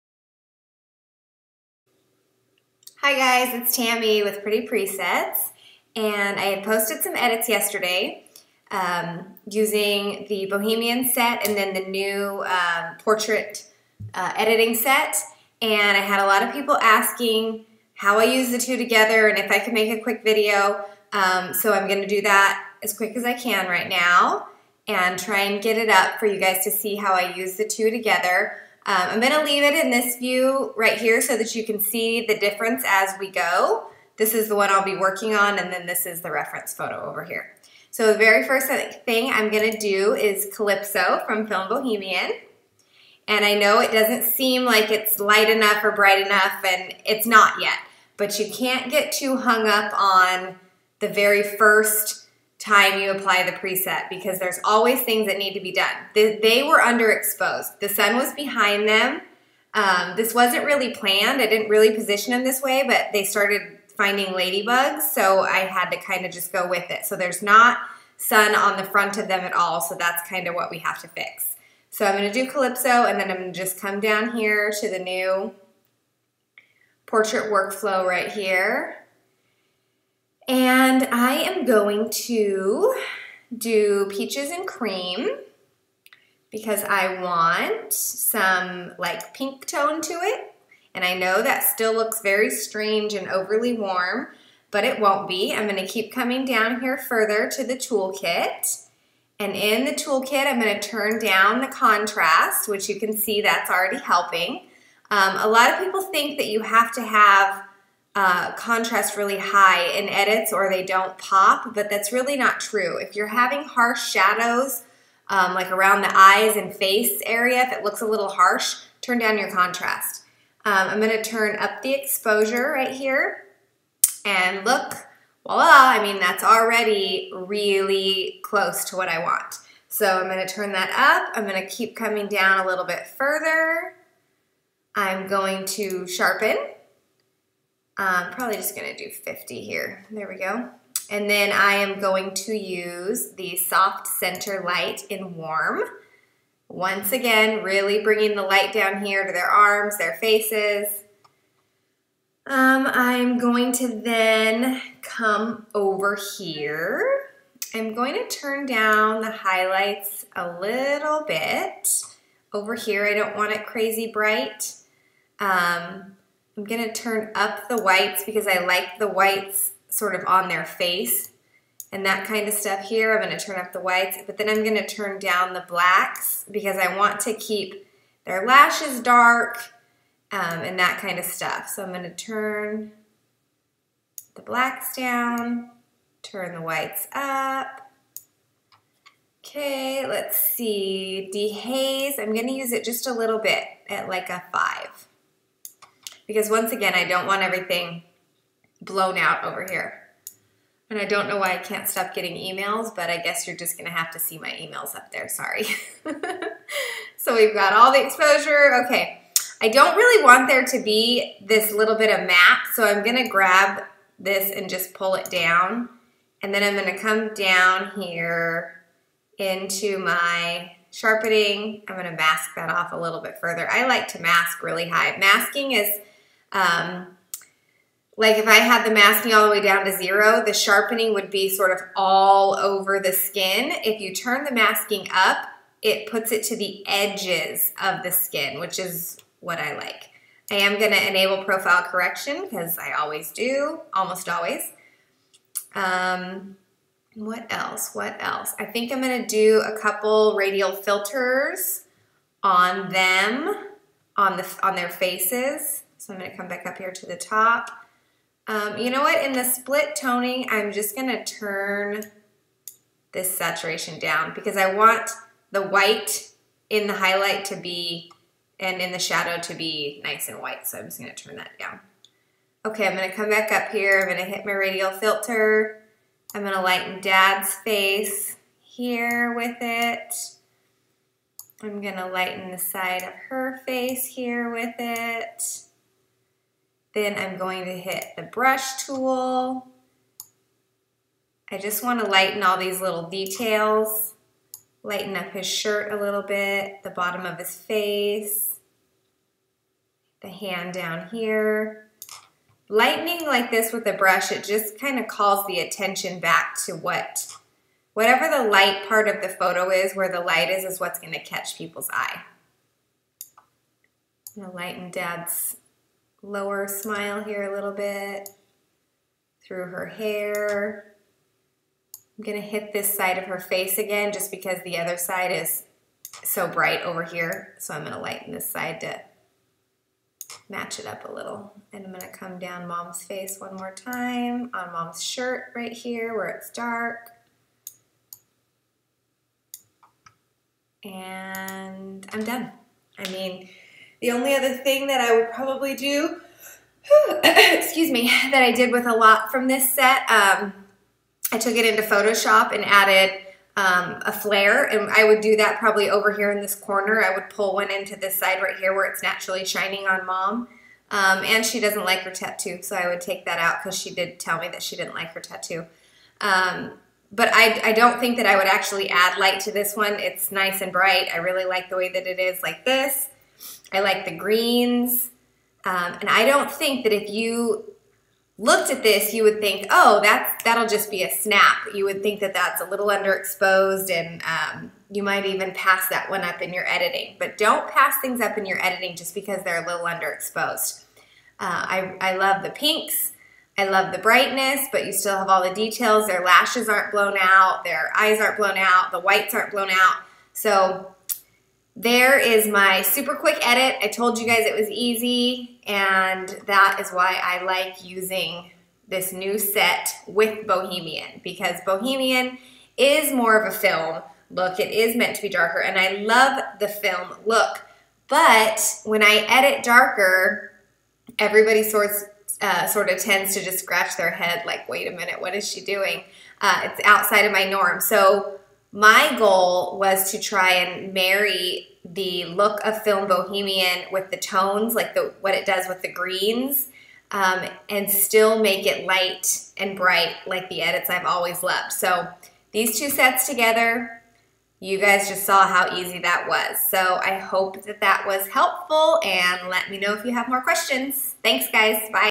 Edit using Film Bohemian along with Clean Edit Portrait Workflow
Editing an underexposed image using Pretty Presets Film Bohemian Collection along with the Clean Edit Portrait Workflow, edited by Tammy Porter.